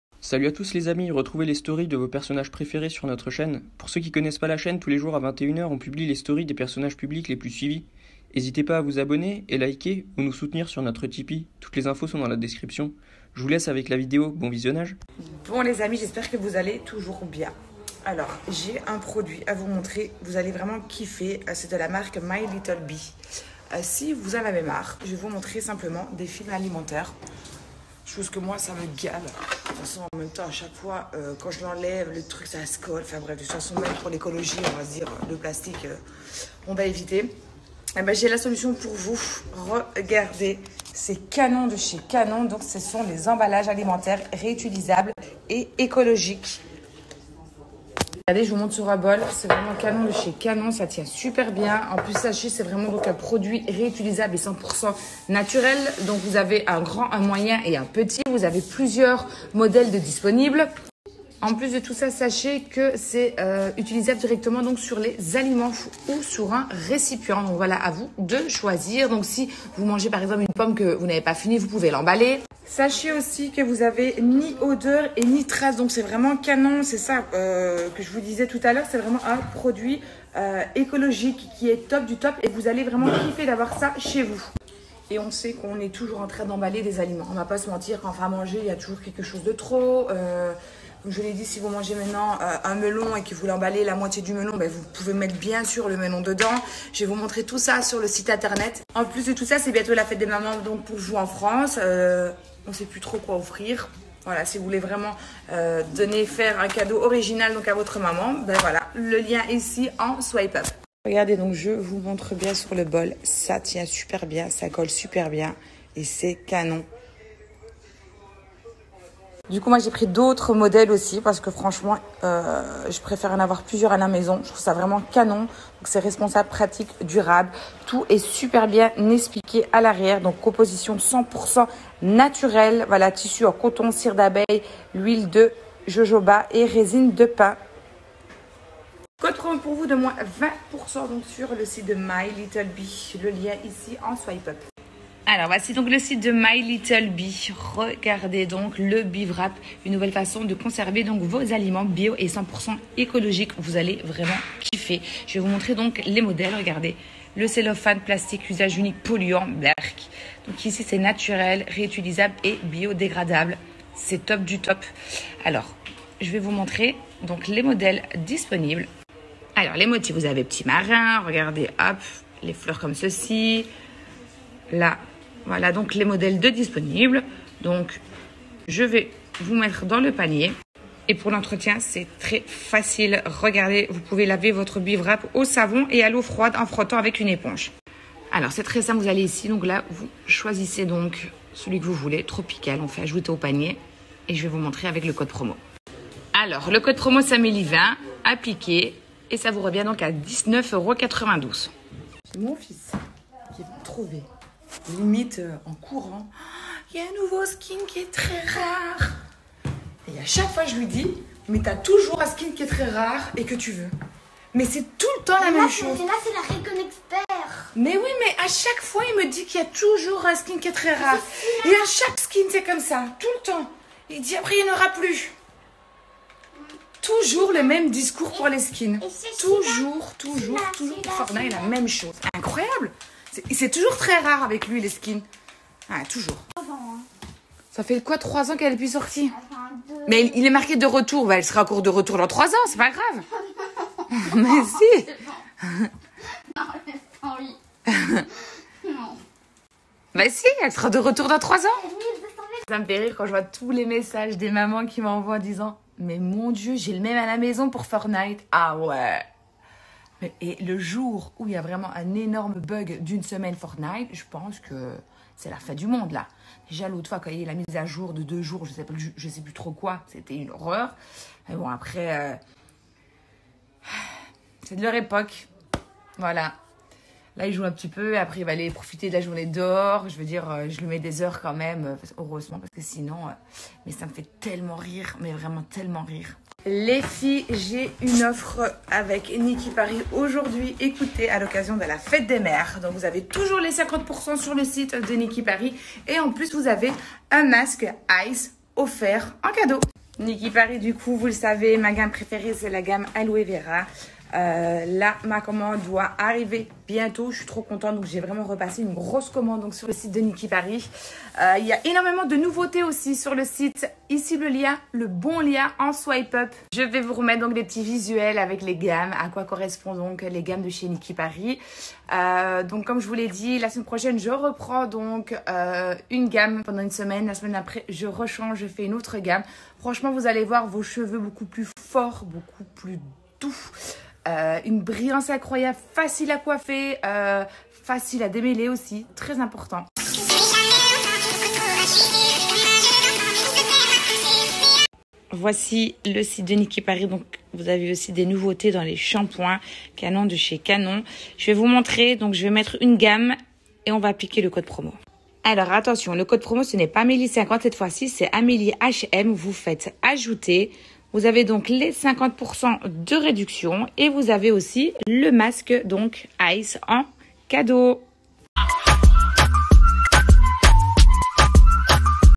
AMELIE NETEN 24H STORY INSTAGRAM - 27 Mai 2020
Revivez en intégralité les Story de Amelie le 27 Mai 2020 - Pour plus de story consultez notre playlist Amelie Neten